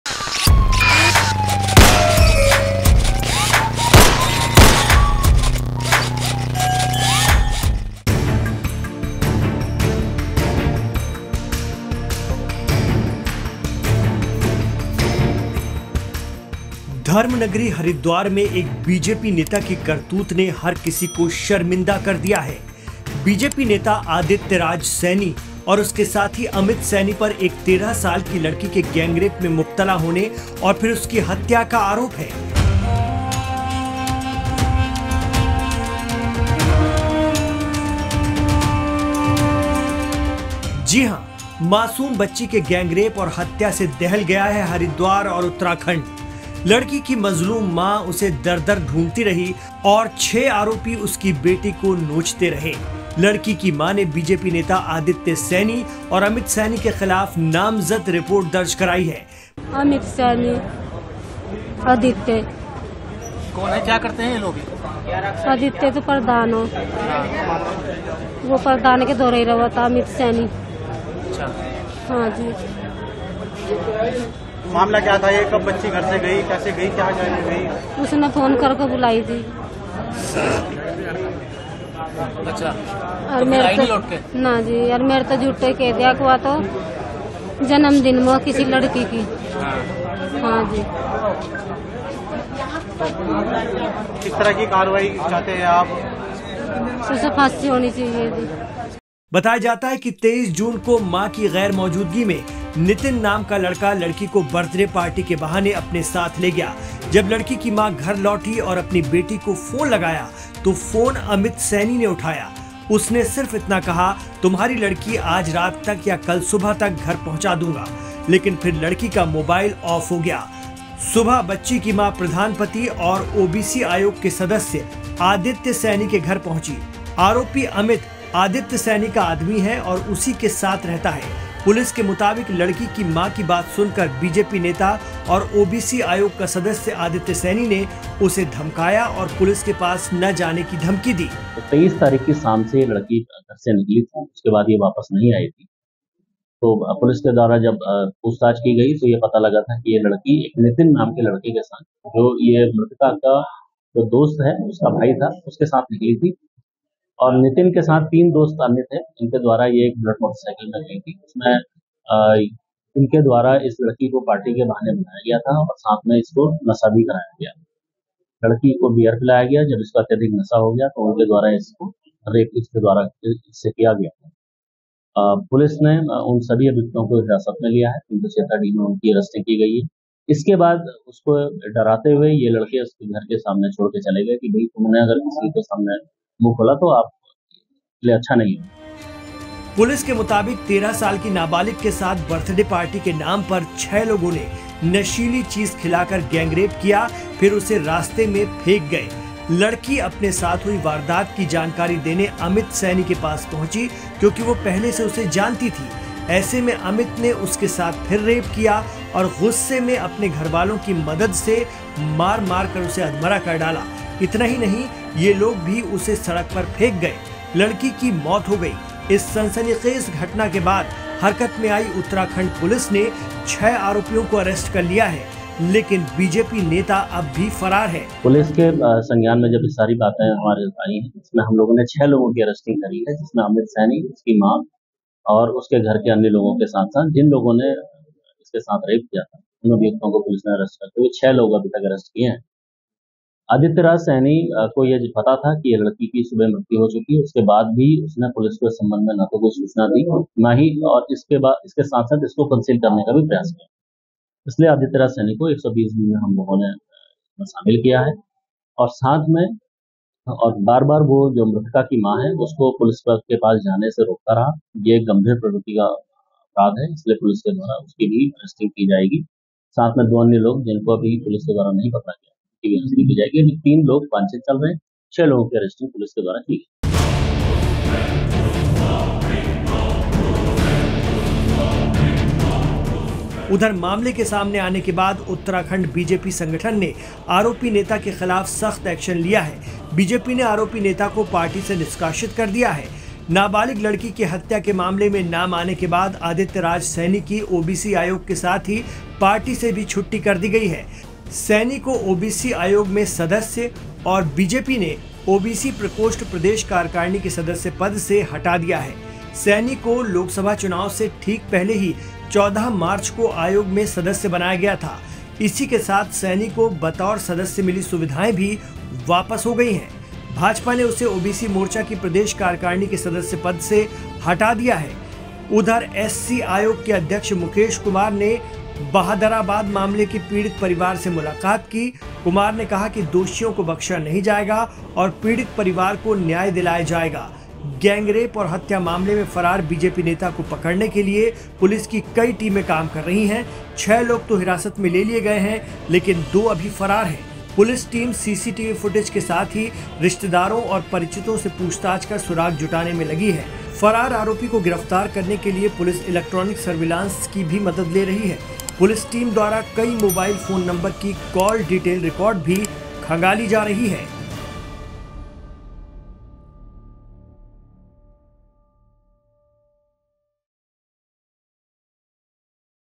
Haridwar Gangrape Case: BJP नेता पर हत्या, Pocso Act में मुकदमा दर्ज, मां ने मांगा इंसाफ।Crime Katha
Haridwar में BJP नेता पर हत्या और Pocso act में मुकदमा दर्ज, माँ ने लगाई इंसाफ की गुहार।Crime Katha

उत्तराखंड के हरिद्वार में एक बीजेपी नेता की करतूत ने हर किसी को शर्मिंदा कर दिया है...। बीजेपी नेता आदित्य राज सैनी और उसके साथी अमित सैनी पर एक 13 साल की लड़की के गैंगरेप केस में मुब्तिला होने और फिर उसकी हत्या का आरोप है. जी हां मासूम बच्ची के गैंगरेप और हत्या से दहल गया है हरिद्वार और उत्तराखंड..लड़की की मज़लूम मां उसे दर दर ढूंढ रही थी और आरोपी उसकी बेटी को नोचते रहे...लड़की की मां ने बीजेपी नेता आदित्य राज सैनी और उसके साथी अमित सैनी के खिलाफ नामजद रिपोर्ट दर्ज कराई है..